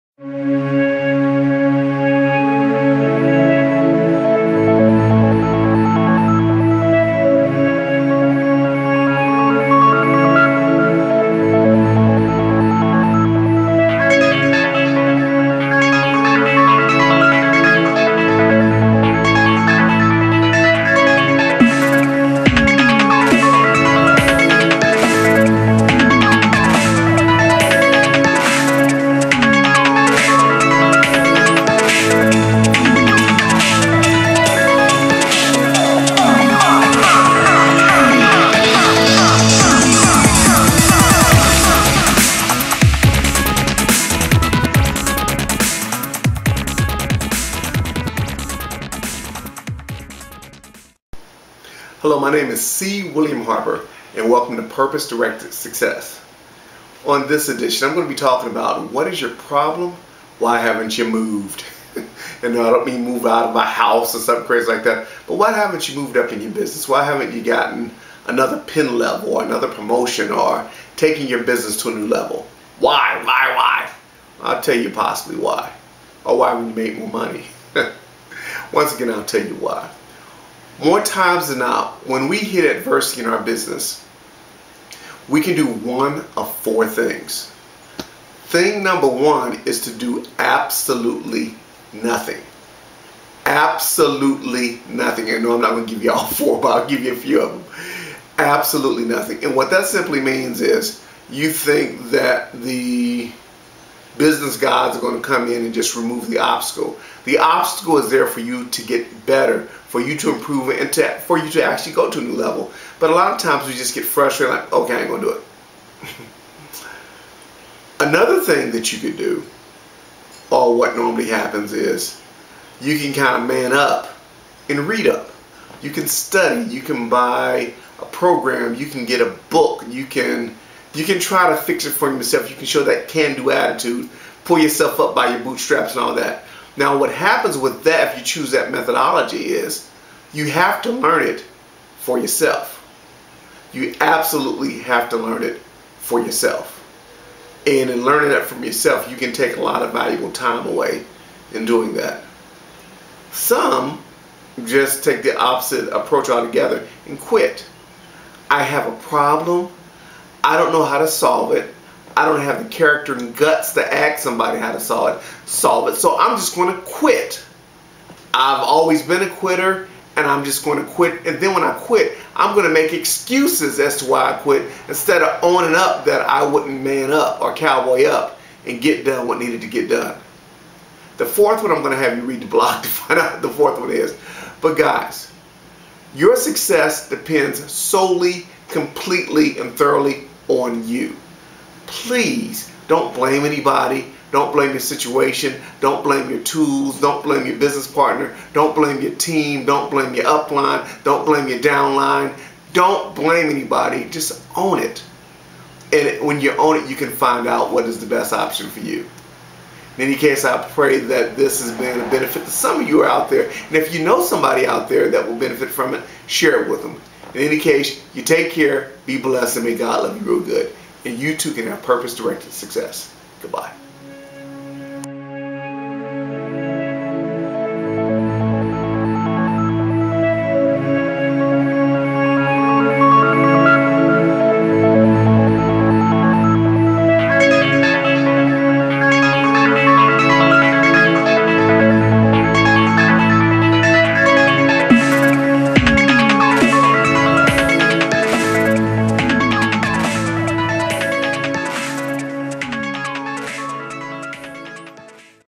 C. William Harper: WHAT IS YOUR PROBLEM? WHY HASN'T YOUR NETWORK MARKETING BUSINESS MOVED?
Sometimes we all need someone in our lives to hold us accountable.  This can serve as me holding you accountable for doing well in your business.  Check out some simple tips that you can utilize to get your business on the right course.